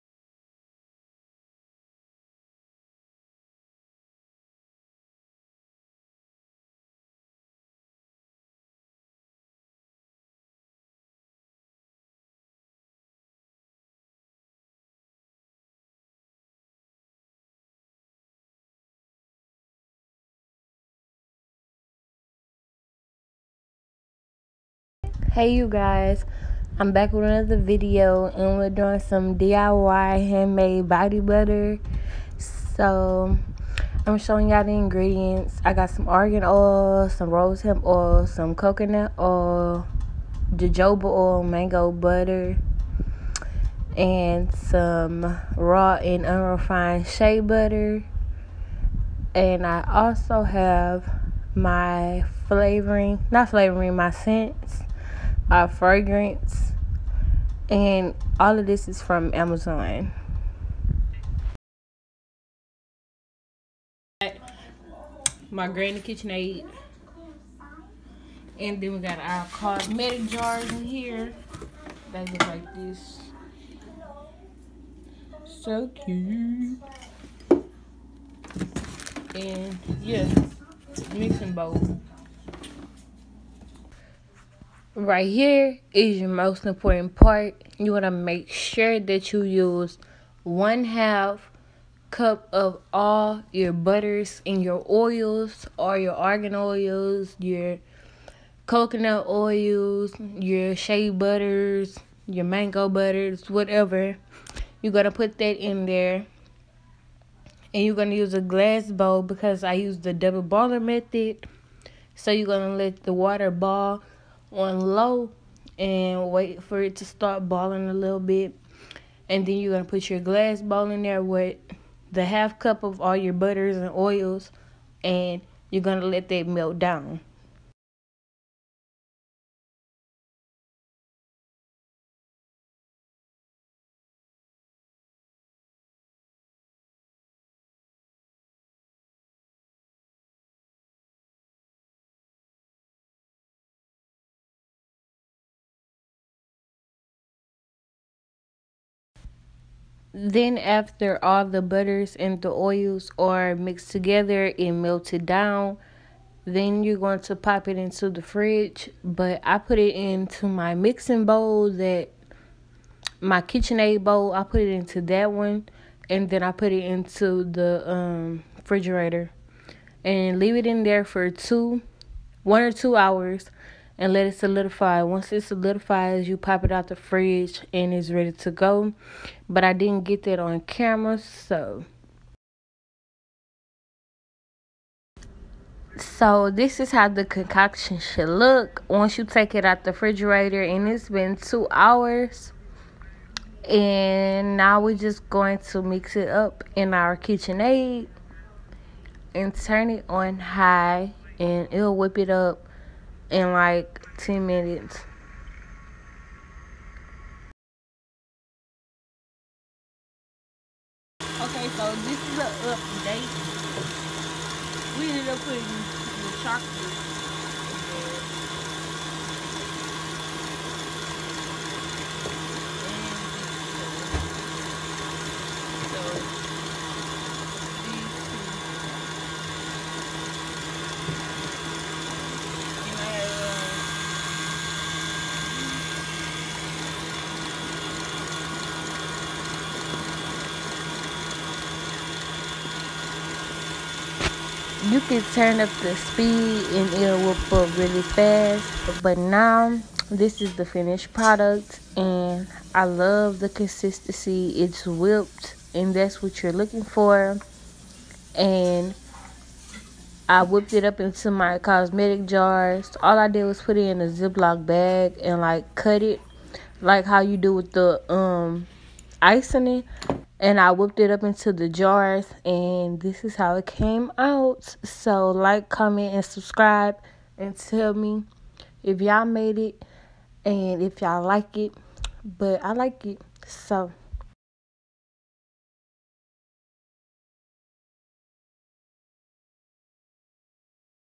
EASY DIY| WHIPPED BODY BUTTER
Hey babes, I’m here with a DIY BODY BUTTER TUTORIAL using all natural products! 🌿

 Shea butter :
Mango butter:
Jojoba oil:
Coconut oil:
Cosmetic jars:

Let me know if you guys like these type of videos !

SHOP my site for virgin hair, wigs & lashes (SALE NOW) ⤵️

Business page: @zaeluxecollection
Main page: @alazaejanay